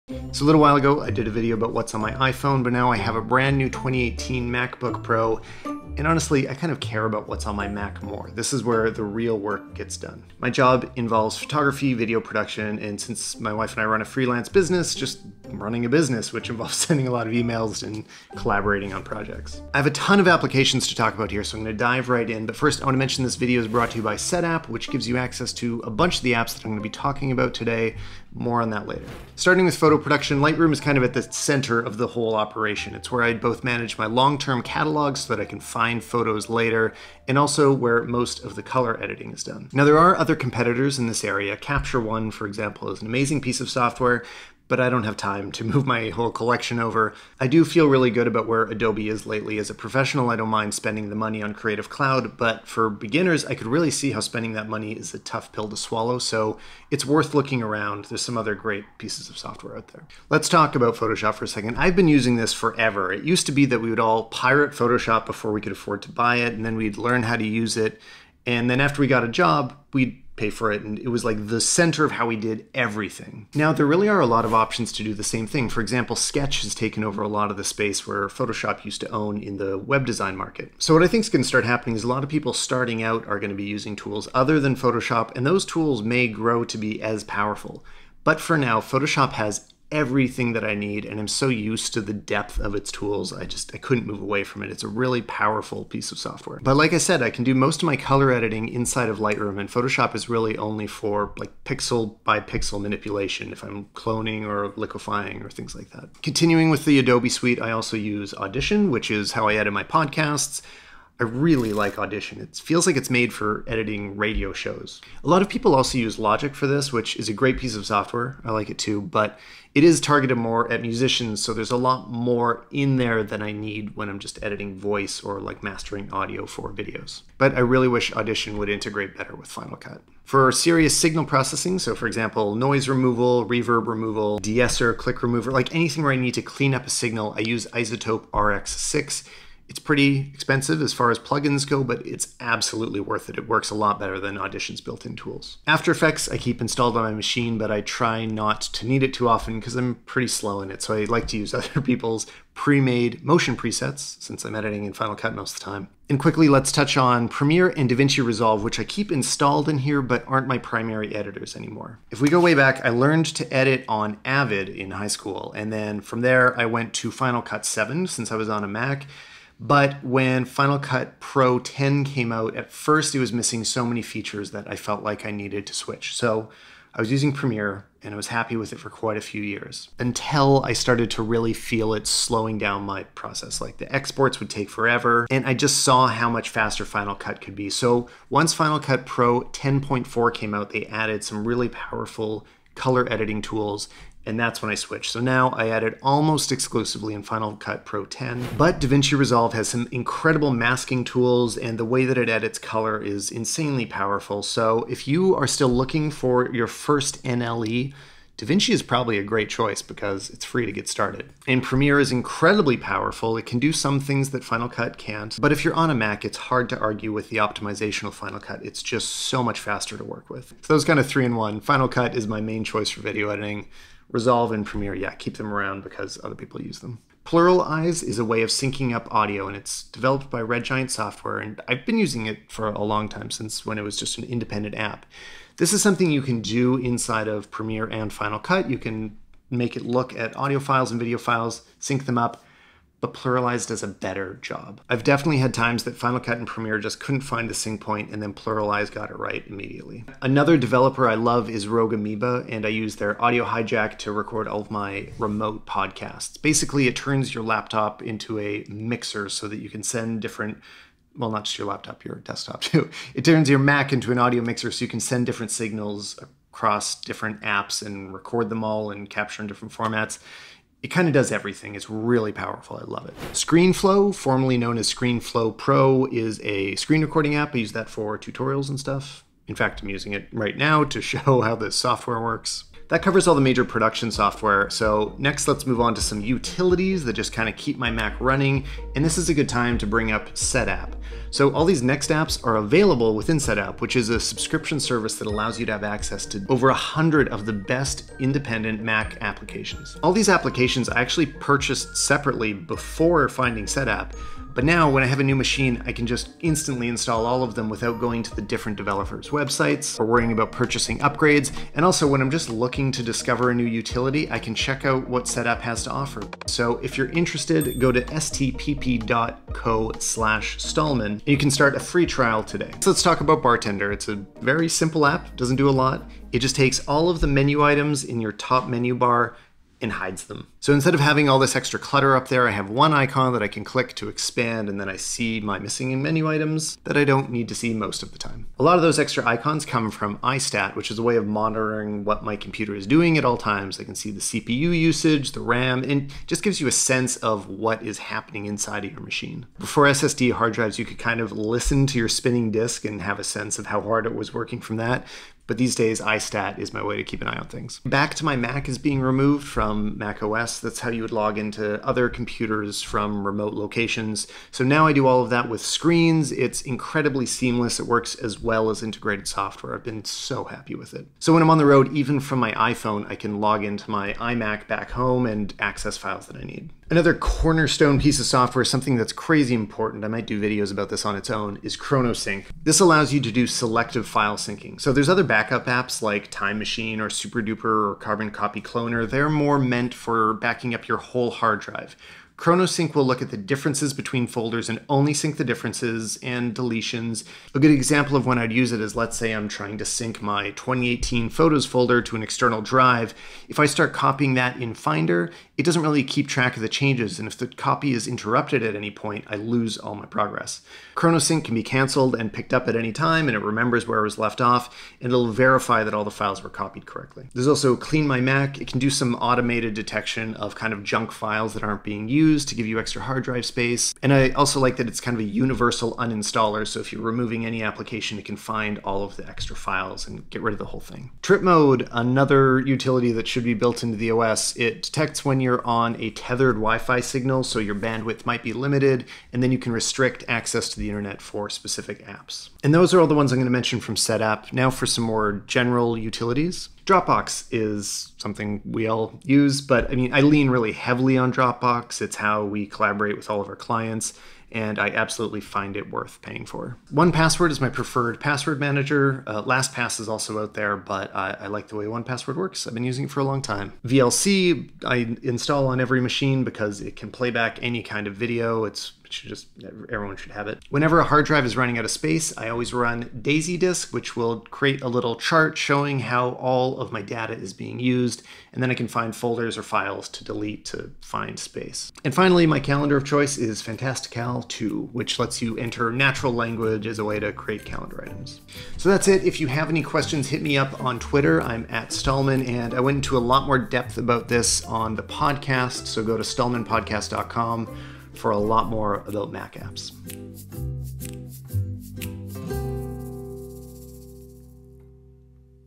What's on my MacBook Pro
The best Mac apps for photographers, video editors and creators
For a list of apps & more details go to the podcast